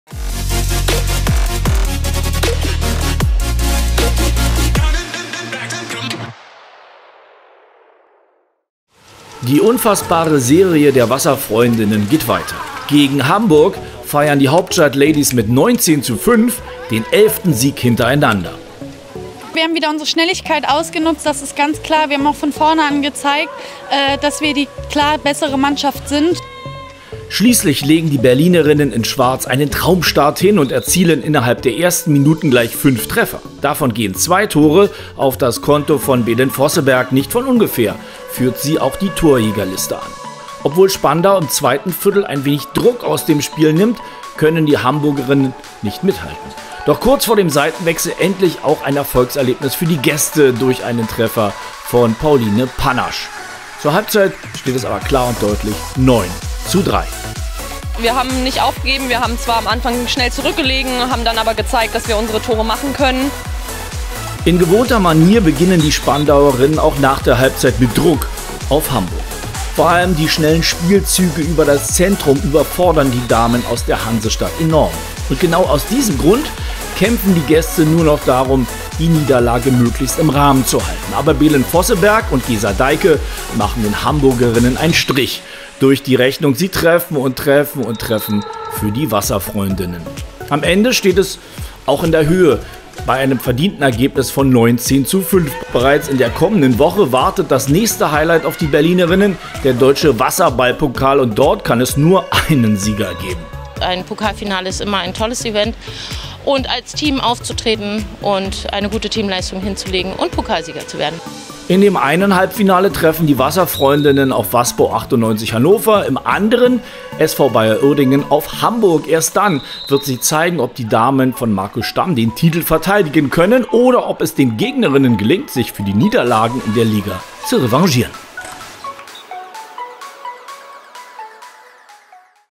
WASSERFREUNDINNEN weiterhin auf SIEGESZUG | Wasserfreundinnen Spandau 04 vs. ETV Hamburg
Die unfassbare Serie der Wasserfreundinnen geht weiter: Gegen Hamburg feiern die Hauptstadtladies mit 19:5 den elften Sieg hintereinander.